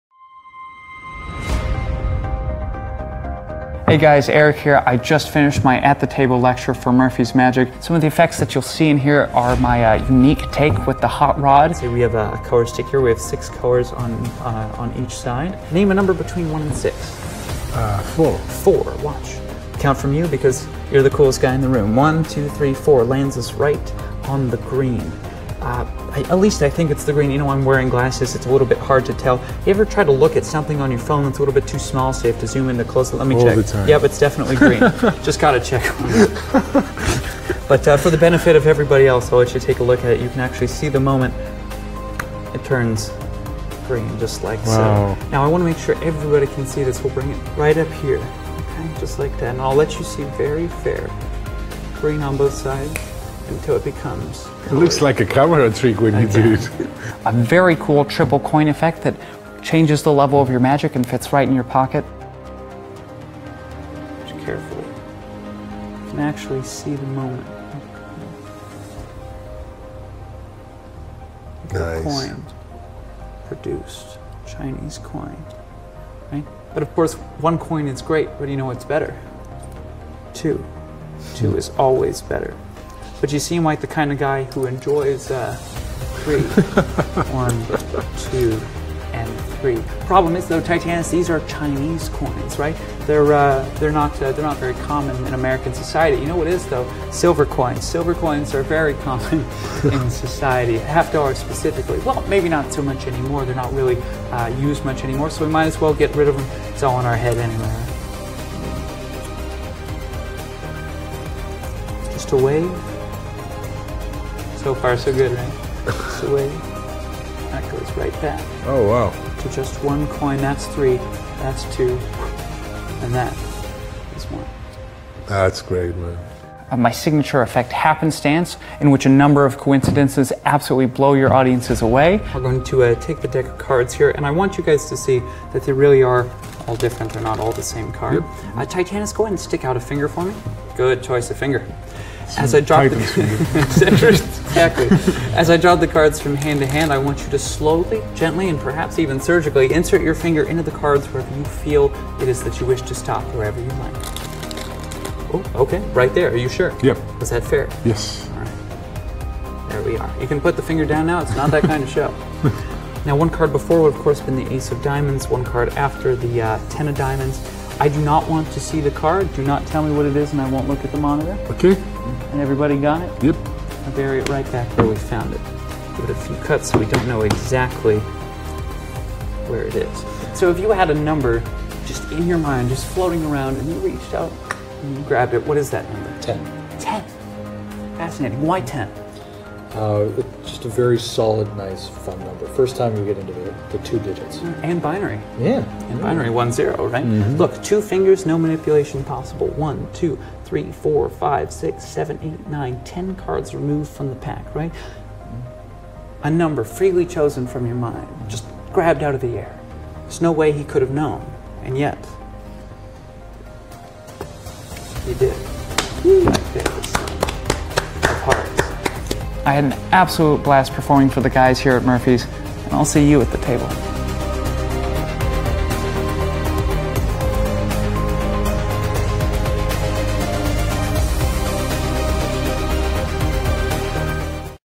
At The Table Live Lecture Eric Stevens January 16th 2019 video DOWNLOAD
"Eric Stevens has all the qualities of a new star of magic. Pay attention to his talents; he is going places!" - Jeff McBride Eric Stevens has been mastering his art for over a decade. He brings his dry wit and smooth magic to his At The Table lecture. With a wide range of effects-from coins to cards and even a little bit of theory sprinkled in-there is something here for everyone. Eric is also very well known for his version of the classic Hot Rod, and if you've got a gimmick collecting dust, this lecture will make you revisit this staple of magic. Come experience this red-hot lecture, right here, At The Table. The One-Armed Man: The audience is taught a little something about the classic 3-Card Monte and how to avoid being swindled - all the while being taken for a ride until the surprise ending. Lazy Bones: Eric's take on the classic Coins Across routine. Hang 'em High: An interactive coin routine that can be paired with Lazy Bones or done as a separate routine. Split-Second Marvel: A freely-chosen time on the stopwatch of a spectator's phone allows for the selection of a card that perfectly matches a previously reversed card in a second deck - one that has been set aside since the beginning. Hot Rod: One of the best Hot Rod routines you will see. Happenstance: The power of coincidence is explored. The magician not only divines the identity of a freely selected card, but influences the participant to name the card's exact position in a shuffled deck. Factory Production: A great production of all four Kings. A Very Dynamic Sandwich: Take any four of a kind and make one of the cards appear inside the Joker sandwich. Takes place in the spectator's hands. Somewhere Out There: The magician is able to predict several spectators' randomly chosen numbers. Tribloon: Three Chinese coins are produced in a very magical way, and one suddenly transforms into a half dollar; finishing with a clean vanish of each coin. Mercury Catch: The spectator selects a card, and the magician is able to snatch the card out of midair as the cards are dribbled to the table. "Eric displays all the creativity, magical skills, natural charm and theatrical experience necessary to become one of the greatest magicians of his generation." - Jay Scott Berry "Eric Stevens has an eye for the details in his magic. He gets me thinking about the way I perform myself and... take my magic to the next level... Eric is a true gent and always a pleasure to watch!" - Tom Elderfield